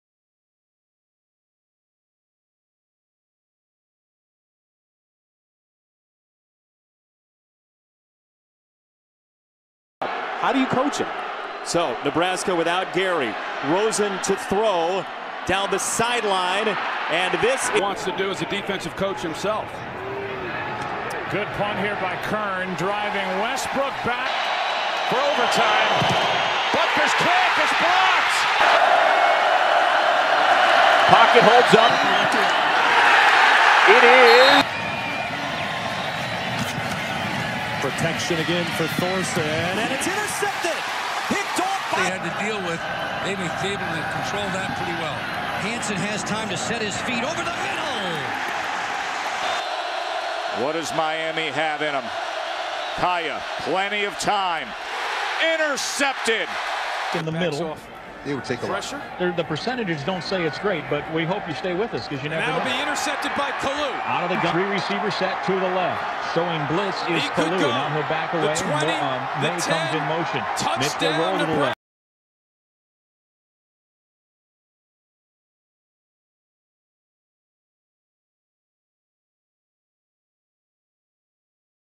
WASHINGTON COMMANDERS: Joshua Kalu ᴴᴰ
BREAKING NEWS || WASHINGTON COMMANDERS || JOSHUA KALU HIGHLIGHTS
44 CAREER GAMES (NFL)
68 TACKLES
2 FORCED FUMBLES
1 INTERCEPTION